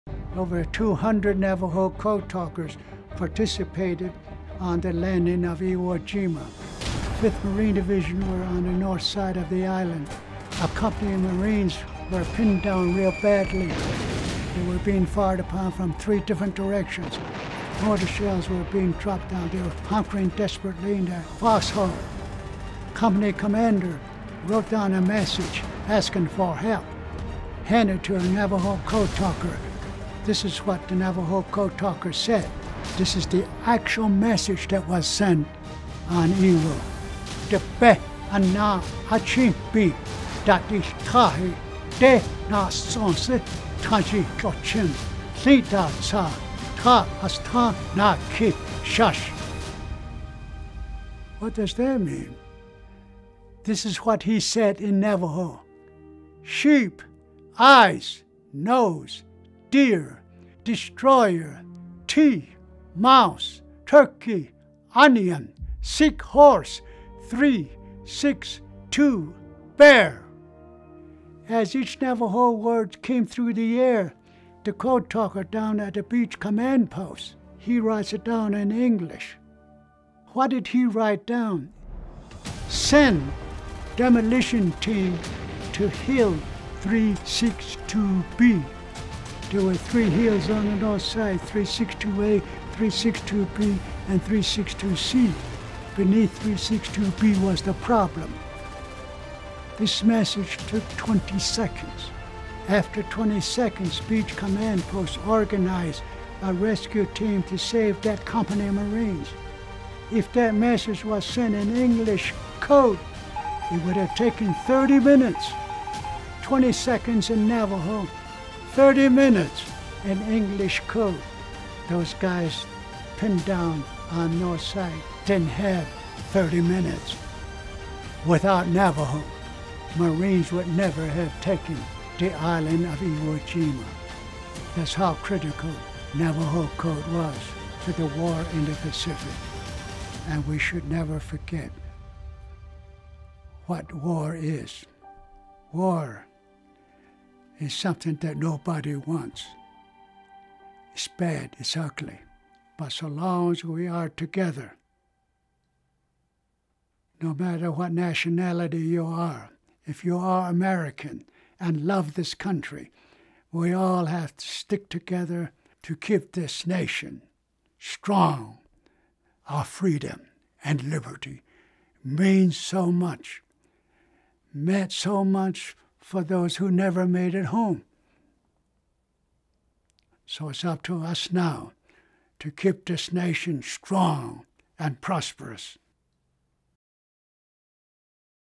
An Unbreakable Code
Credit: Reynaldo Leal | Date Taken: 02/19/2020
Peter MacDonald Sr. is one of the last remaining Navajo Code Talkers. On the 75th anniversary of the Battle of Iwo Jima, he explains what made the “unbreakable” code so effective, and how it helped save lives and secure victory in the Pacific. An Unbreakable Code.